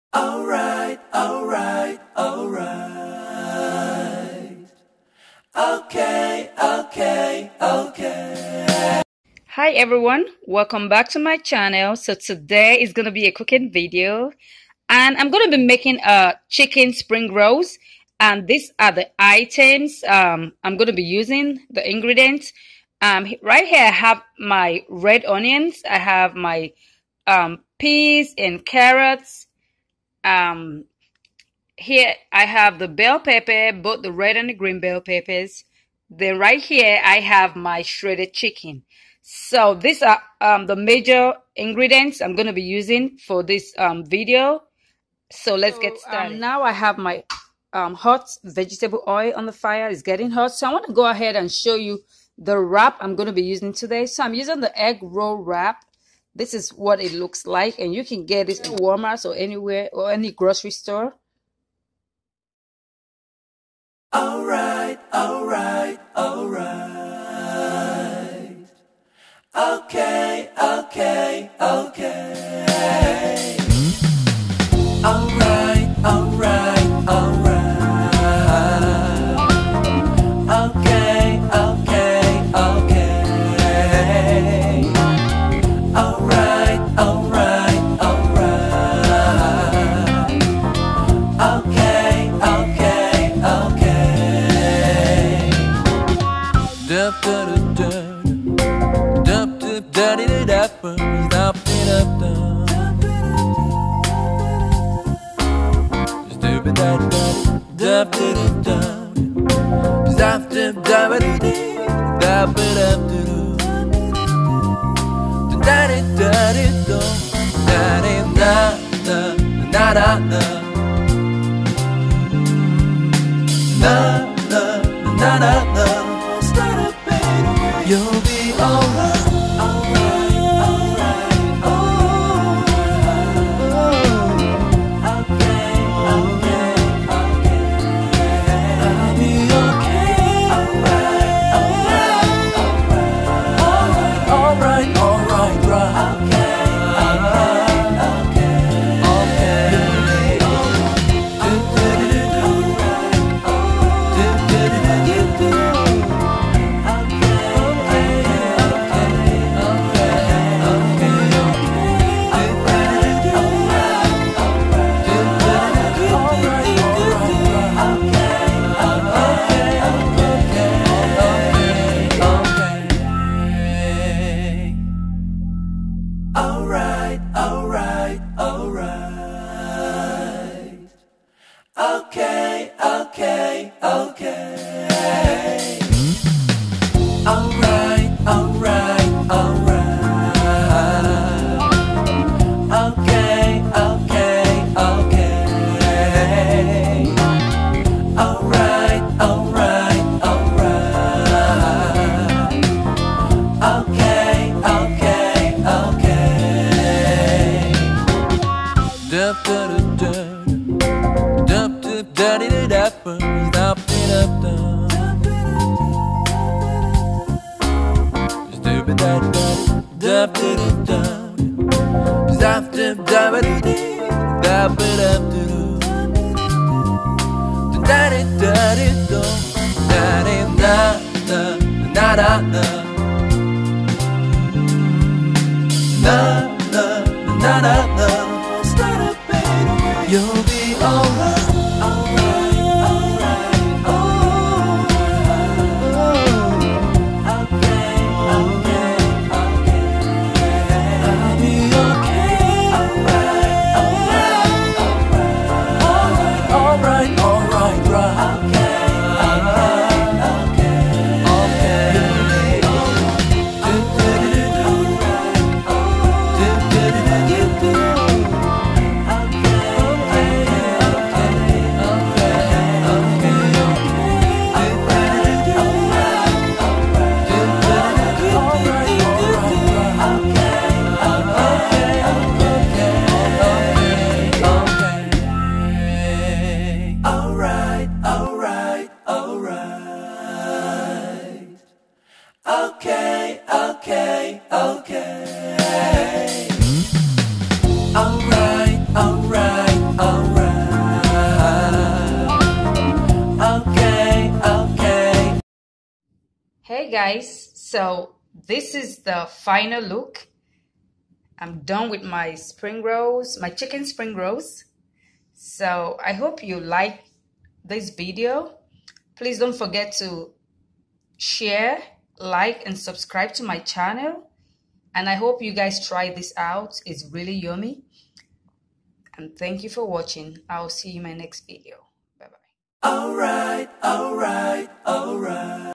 DIY/ How to make vegetable chicken Spring rolls
Are you looking at trying out easy chops? Well this video got you covered! Easiest spring rolls ever!

Itels/ Ingredients:-
Vegetable oil
Shredded chicken
Red onions
Spring onions
Carrots
Peas
Bell peppers red & green.

Seasonings:-
Chicken powder/ Maggi
Nutmeg
Curry
Thyme
Garlic
Dry pepper
Salt.
Please don't forget to give me a thumbs up 👍👍👍.
I'll see you in my next video, have a great day😍🥰😘

Have you watched my previous video on how to make a healthy fish vegetable soup?